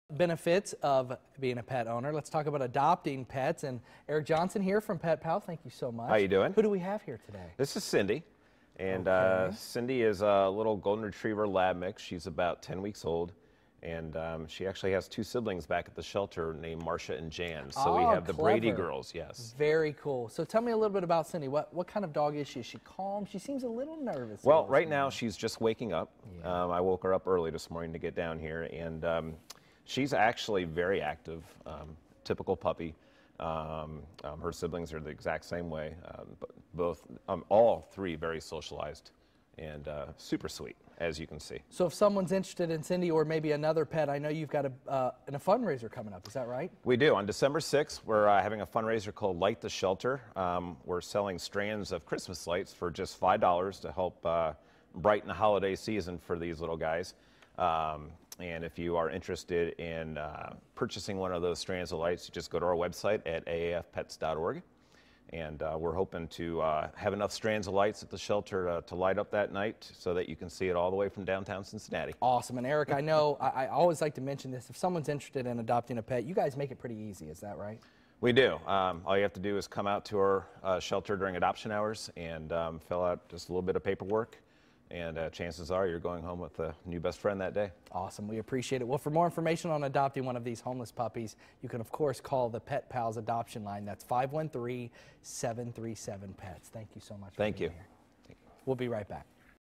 Pet Pals
Meet Cindy, a 10-week-old lab and golden retriever mix in need of a home.